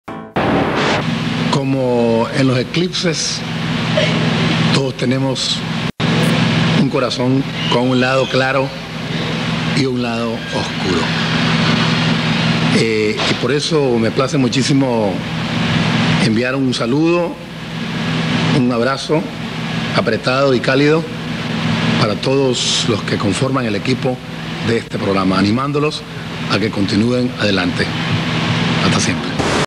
19añosLadoOscuroTn8 Saludo de Carlos Mejía Godoy
El cantautor nicaragüense Carlos Mejía Godoy saluda a Lado Oscuro de TN8